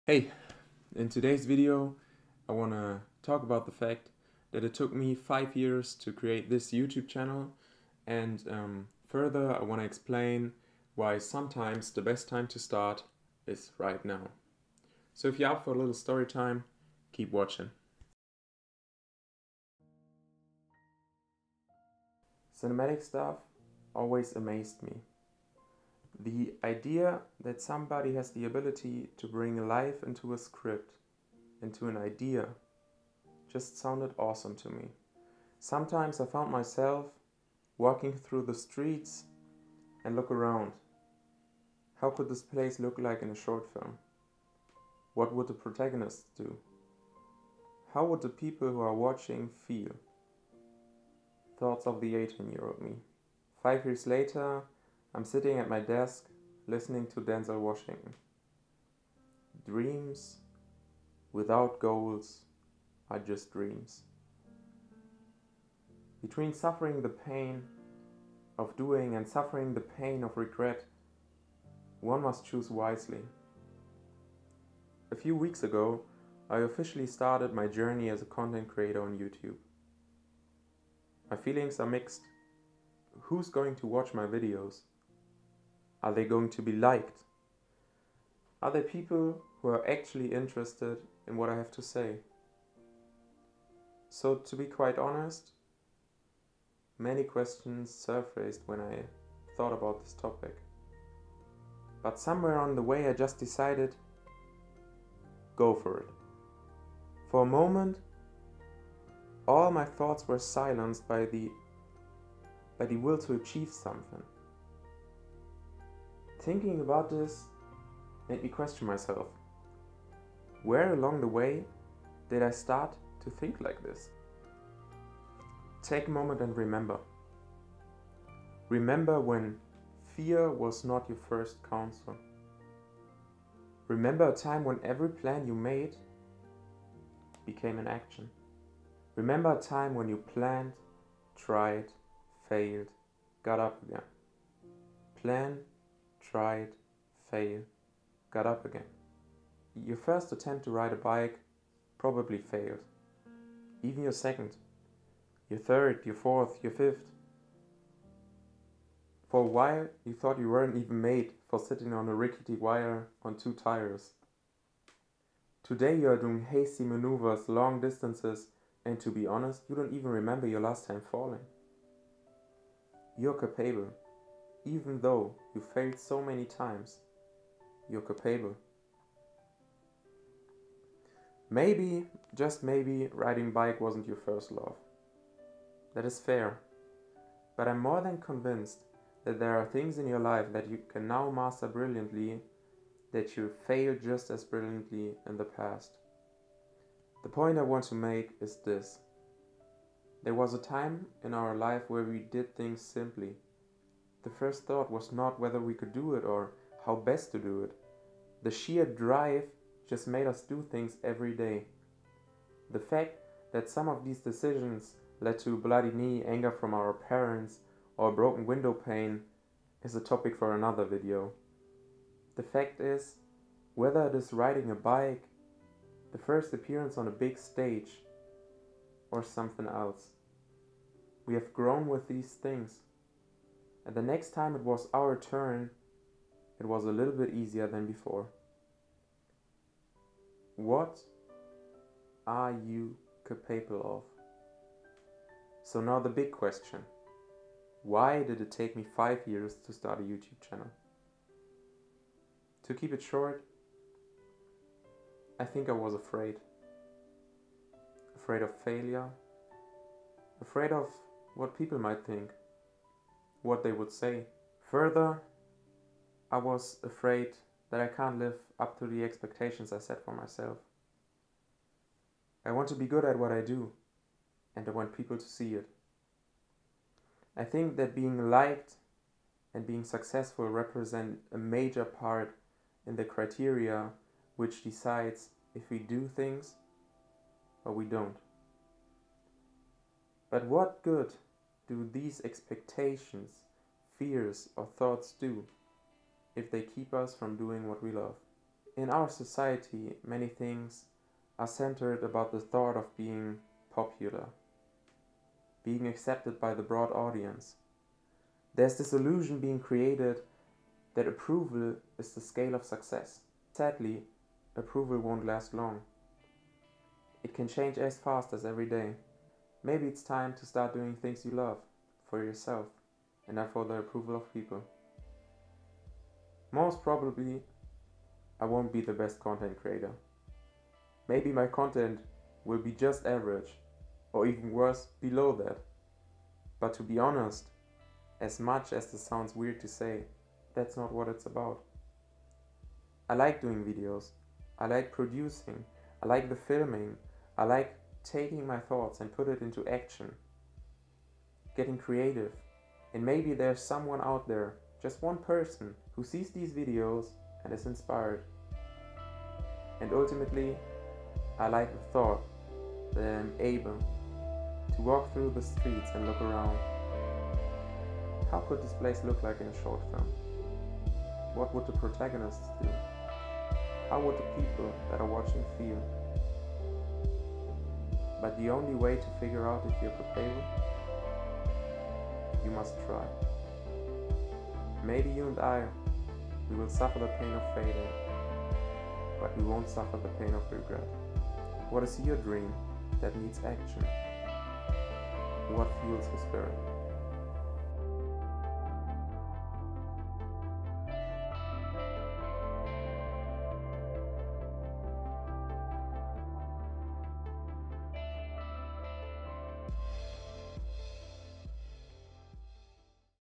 START NOW | 5 years to create a YouTube Channel? | John's Story
Hey there! In todays video I talk about why sometimes it takes us ages to start something we actually love to do. We get caught up, are out of motivation or simply are full of fear about what others might say. But dreams are meant to be lived and at some point we gotta make a goal and aspire to achieve it.
Everyone of us fails, we still need to try.